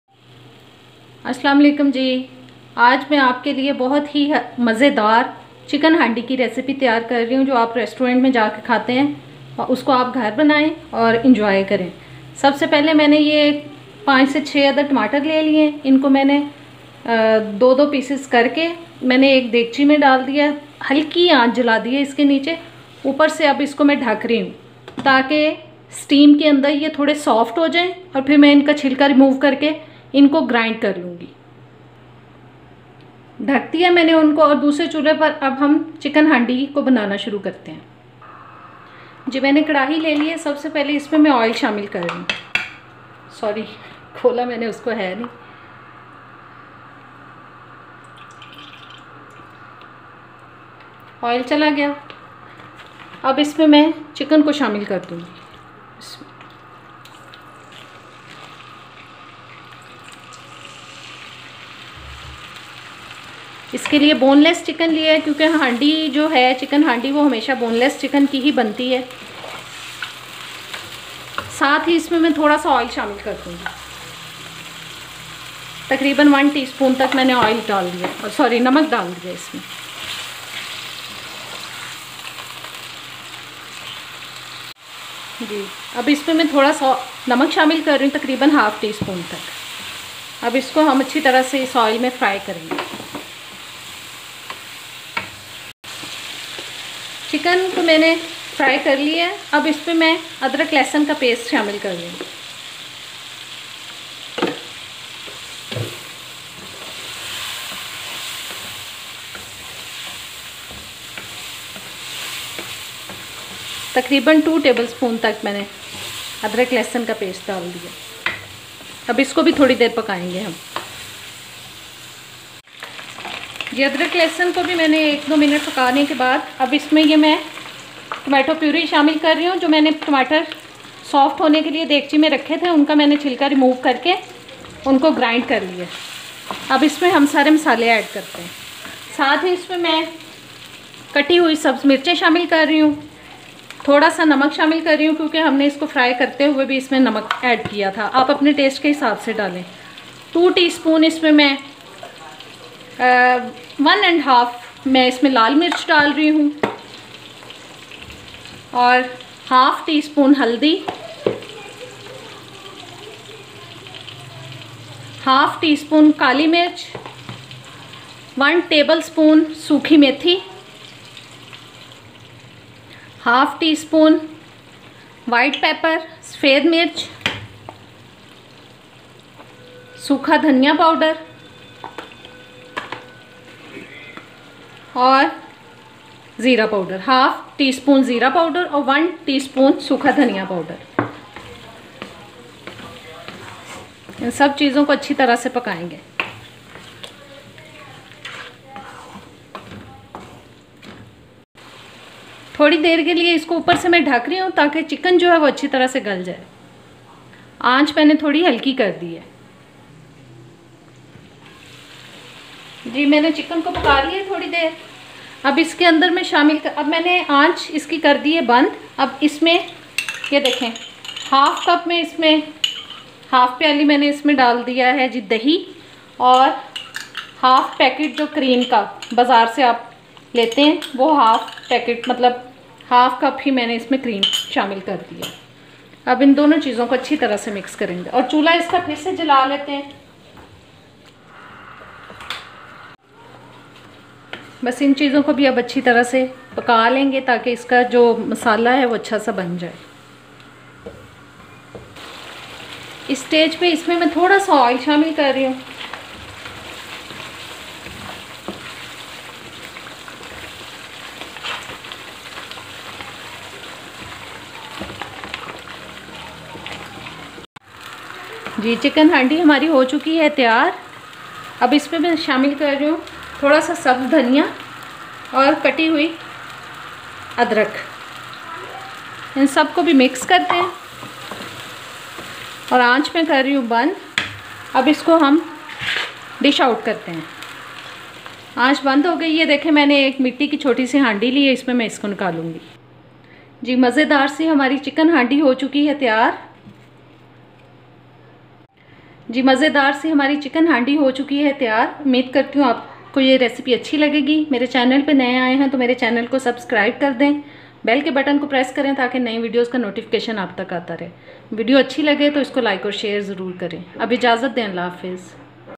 Lajawab Chicken Handi Restaurant Style Recipe In Urdu And Hindi
ingredients

oil   1/3cup
chicken boneless   1/2kg
red chilli powder   1-1/2ts
black pepper   1/2ts
dry fenugreek   1tbs
white pepper   1/2ts
cummin seeds powder   1/2ts
coriander powder   1-1/2ts
yogurt   1/2cup
cream   1/2cup
fresh coriander   2tbs
ginger pieces   1tbs

Healthy Diet With Fasiha My New Channel Link